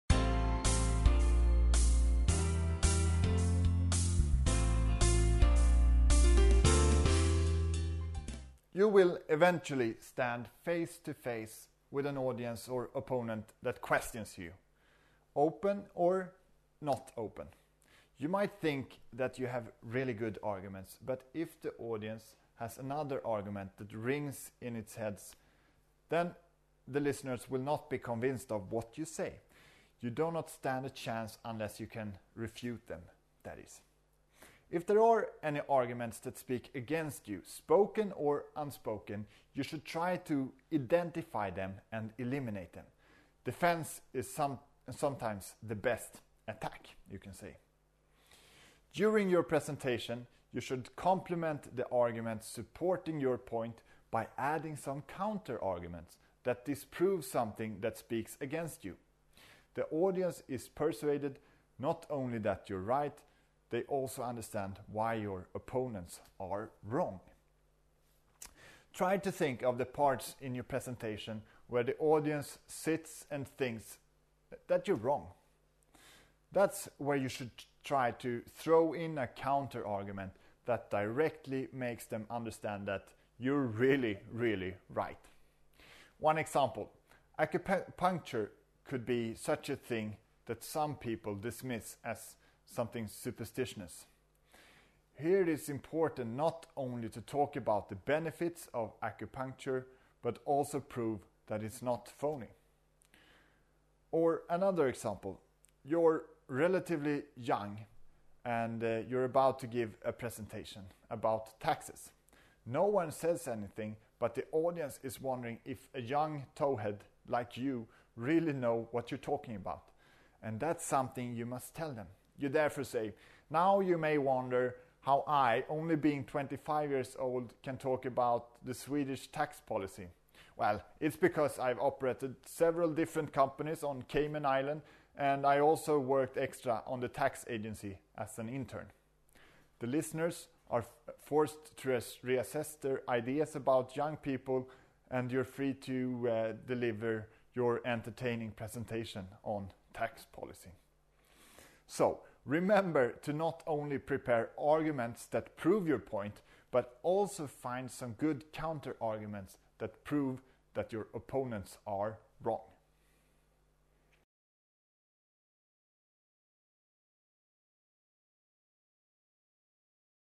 1.2.11 Counter Arguments
Make sure to prepare some good counter arguments in case your opponents have decided to prove you wrong. The counter argument will then act as your well needed defence.